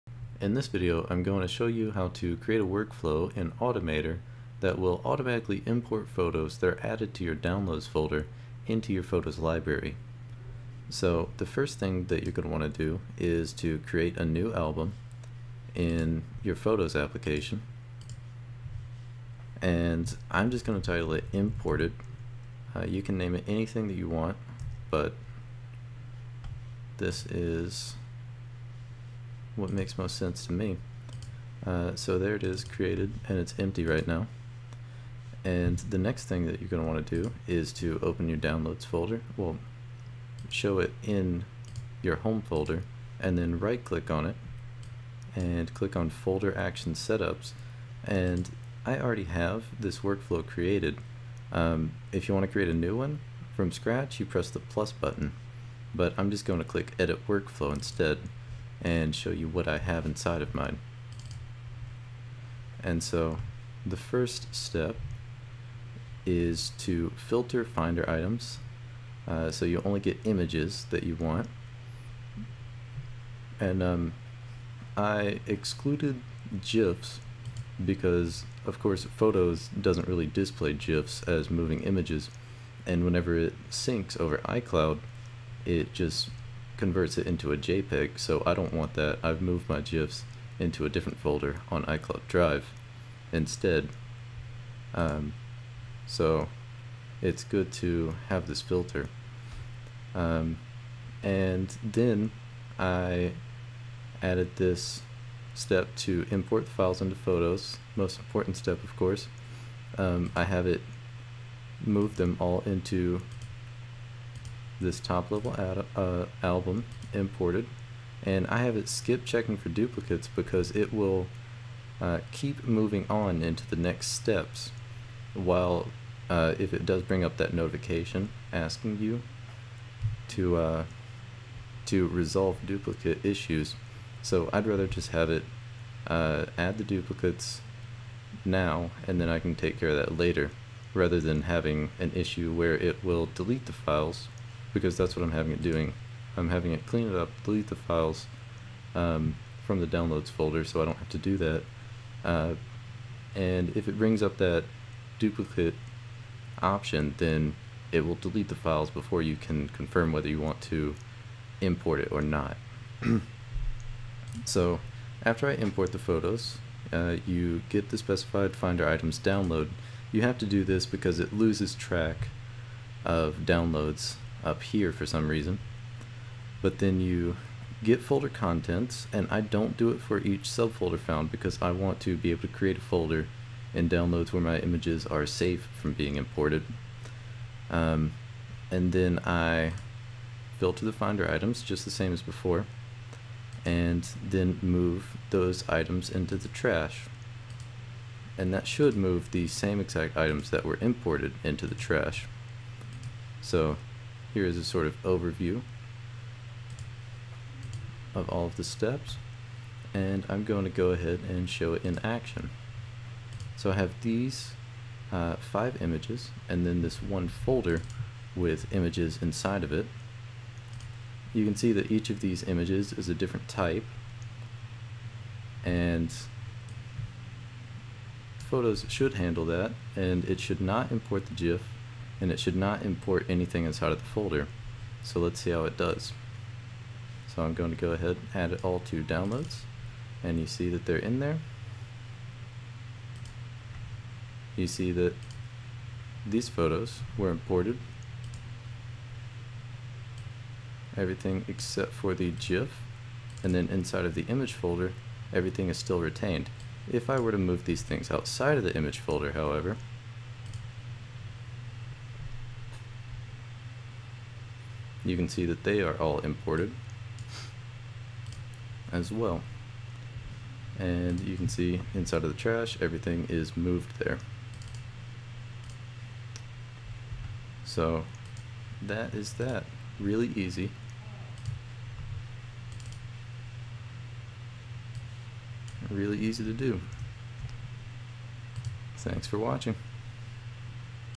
Tutorial for Mac - Import Photos Automator Workflow
This is a tutorial on how to create a workflow in Automator to automatically import photos that are added to your Downloads folder (or any folder) into your Photos library. This was done on OS X El Capitan 10.11.4.